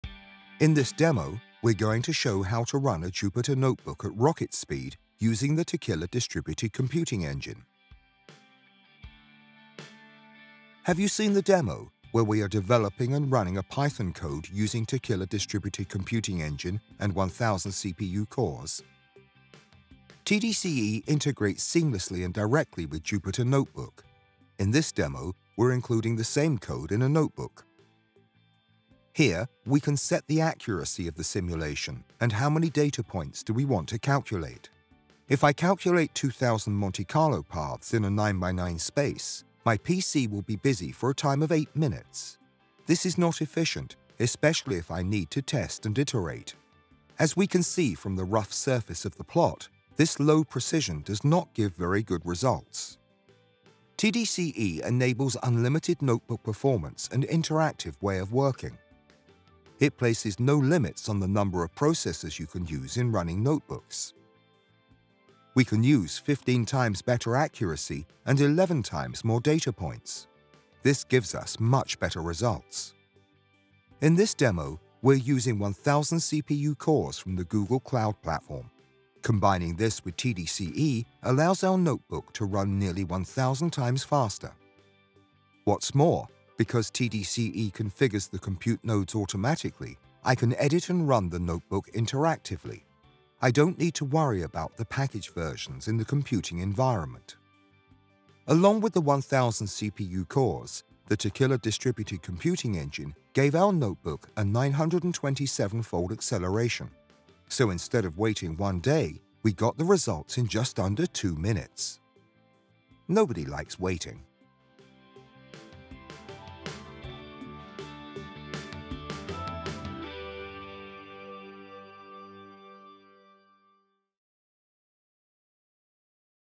1000x speed to Jupyter Notebook using Techila Distributed Computing Engine
This demo shows how a Jupyter notebook can run at rocket speed using Techila Distributed Computing Engine.

Techila Distributed Computing Engine TDCE is an interactive big computing solution that enables highly scalable notebook runs with simplicity.

The solution integrates seamlessly and directly with Jupyter Notebook (also known as IPython Notebook). The easy functions in the Techila package enable interactive parallel Python, R programming language and Julia language runs with simplicity. You can develop, test and iterate efficiently.

More information about Techila package for Python, R programming language and Julia computing

In this demo we’re using 1000 CPU cores. Combining this with Techila’s technology it allows our notebook to run almost 1000 times faster.